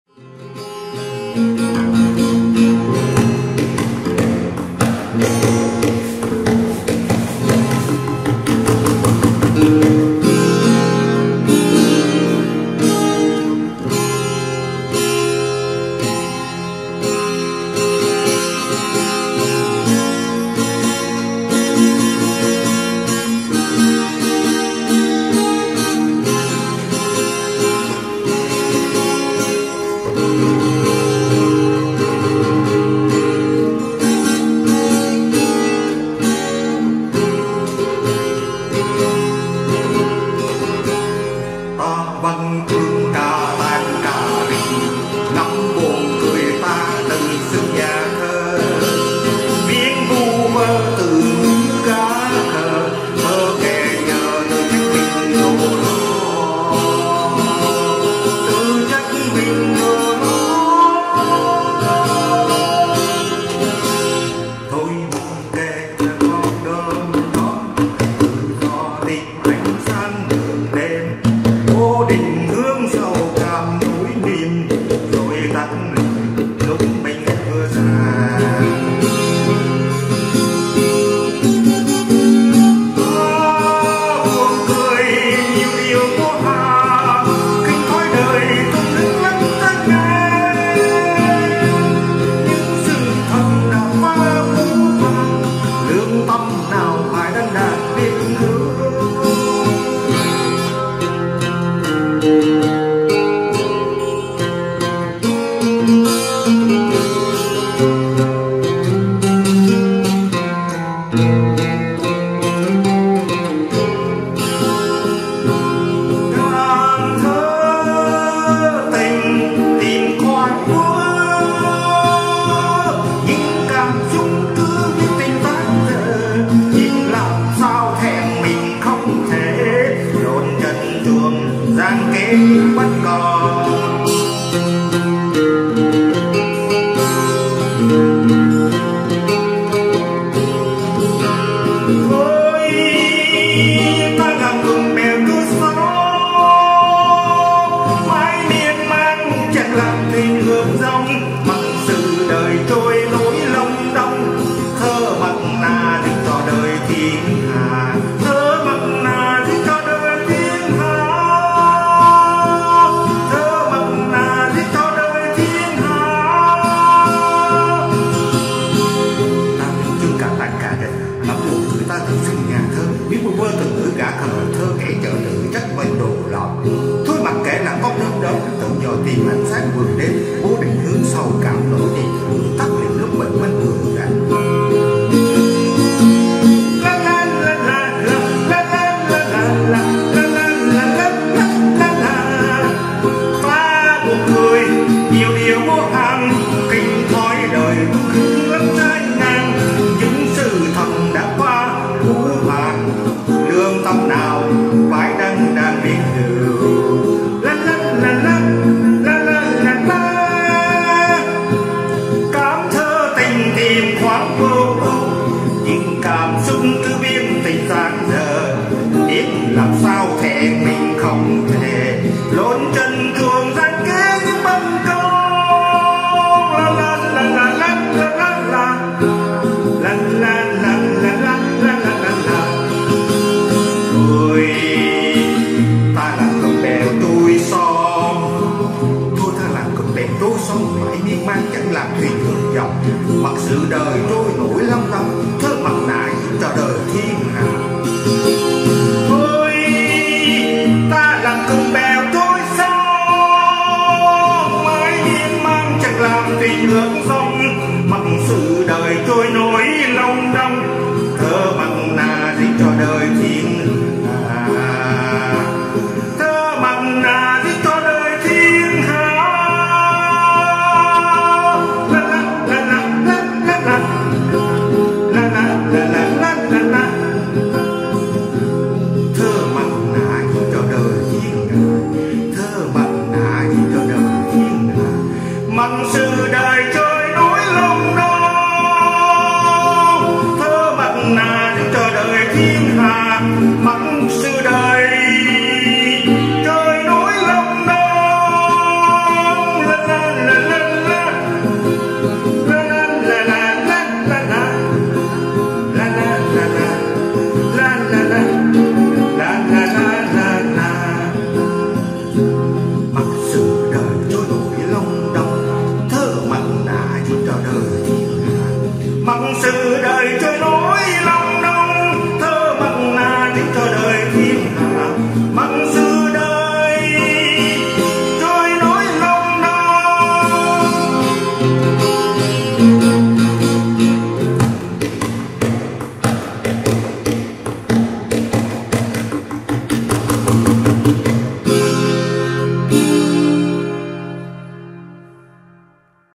[FULL HD]YÊU GÃ KHỜ LÀM THƠ ||J'AIME LE TYPE STUPIDE QUI ÉCRIT DE LA POÉSIE||HOÀI VŨ
NHÀ THƠ HOÀI VŨ
TÁC PHẨM:YÊU GÃ KHỜ LÀM THƠ
||J'AIME LE TYPE STUPIDE QUI ÉCRIT DE LA POÉSIE||
THƠ:HOÀI VŨ
(YÊU GÃ KHỜ LÀM THƠ)
THƠ...MẶT NẠ
Ta văn chương cà tàng, cà rịch
Ngẫm buồn cười ta tự xưng Nhà Thơ
Viết vu vơ từ ngữ...gã khờ
Thơ kẻ chợ...tự trách mình đồ lỏm
Thôi mặc kệ ! Làm con đom đóm
Tự dò tìm ánh sáng vườn đêm
Vô định hướng...sầu cảm nỗi niềm
Rồi tắt lịm lúc bình minh vừa rạng
Ta buồn cười nhiễu điều vô hạn
Khinh thói đời...tung hứng lắm trái ngang
Những sự thật đã quá phũ phàng
Lương tâm nào phải đăng đàn viết hữu
Cảm thơ tình tìm khoảng vô ưu
Nhưng cảm xúc cứ viết tình dang dở
Biết làm sao ! Thẹn mình không thể...
Lột trần truồng gian kế, nhũng, bất công...
Thôi ta làm cụm bèo trôi sông
Mãi miên man, chẳng làm thuyền ngược dòng
Mặc sự đời trôi nổi long đong
Thơ mặt nạ diễn trò đời...thiên hạ.
                                                          Hoài Vũ
TRADUCTION FRANÇAISE
POÉSIE...MASQUES
Je suis un écrivain de potins et de potins
C'est drôle de penser que je me considère comme un poète
Écrire des mots au hasard... idiot
La poésie du poète... se reproche d'être un menteur
Pas grave! Soyez une luciole
Détecter automatiquement la lumière dans le jardin la nuit
Sans direction... tristesse et tristesse
Puis il s'est éteint à l'aube
Je ris de choses infinies
Dédaigner la vie... jongler est très contraire
Les faits étaient trop durs
Quelle conscience devrait poster un forum pour écrire ?
Ressentir la poésie amoureuse, chercher un moment sans soucis
Mais mes sentiments continuent d'écrire un amour inachevé
Comment je sais ! Je suis désolé, je ne peux pas...
Dénoncer la fraude, la corruption, l'injustice...
Eh bien, créons un groupe de jacinthes d'eau pour flotter sur la rivière.
Pour toujours, pour toujours, ne fais pas de bateau en amont
Laisse la vie passer
Les masques de poésie jouent le jeu de la vie... du monde.
                                                           Hoai Vu
Đường liên kết của video